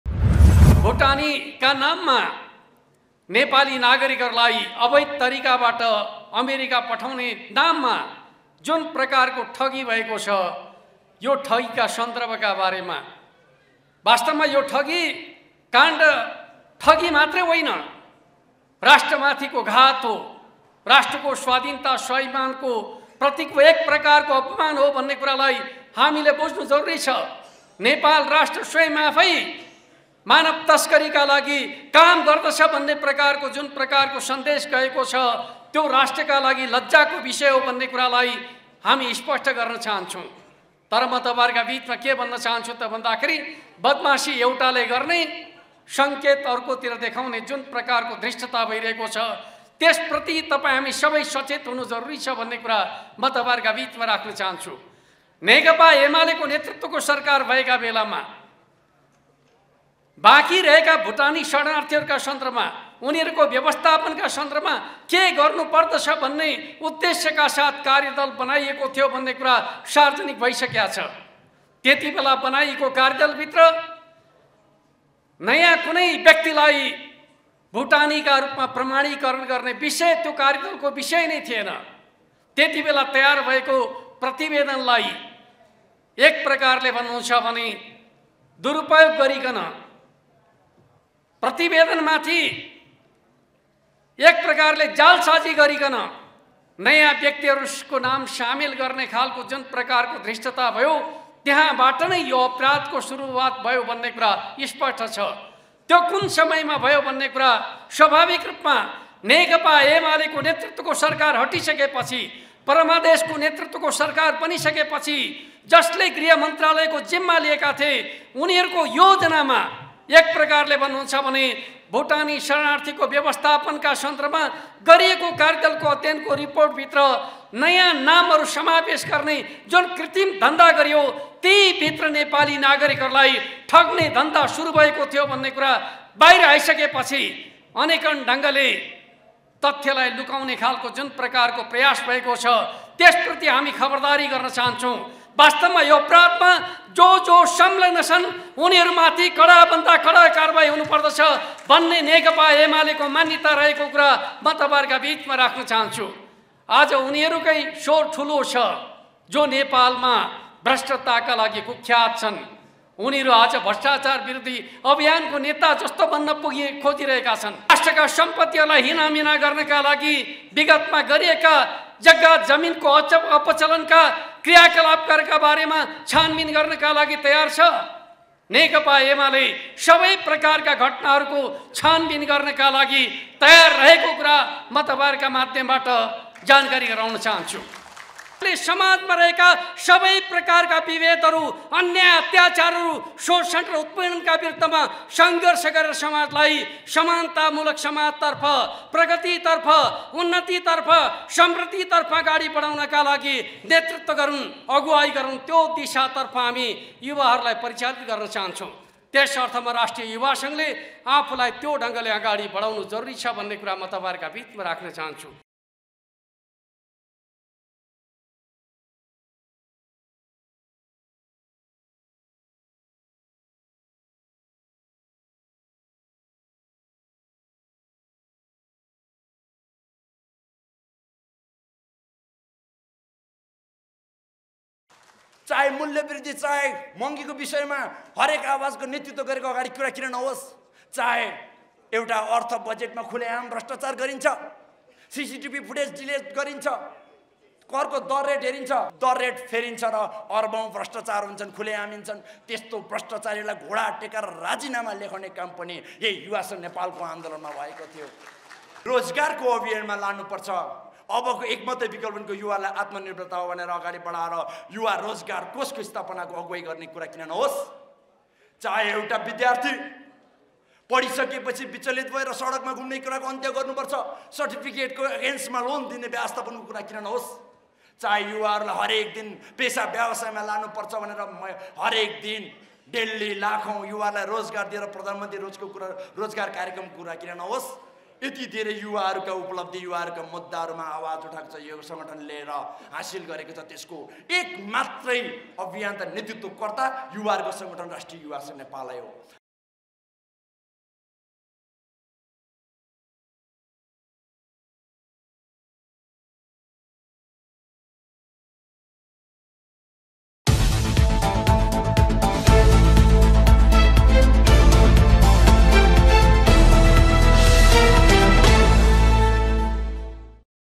नेकपा एमालेका महासचिव शंकर पोखरेलले नक्कली भुटानी शरणार्थी प्रकरण राष्ट्रमाथिको घात र अपमान
नेकपा एमालेका महासचिव शंकर पोखरेलले नक्कली भुटानी शरणार्थी प्रकरण राष्ट्रमाथिको घात र अपमान भएको बताउनुभएको छ ।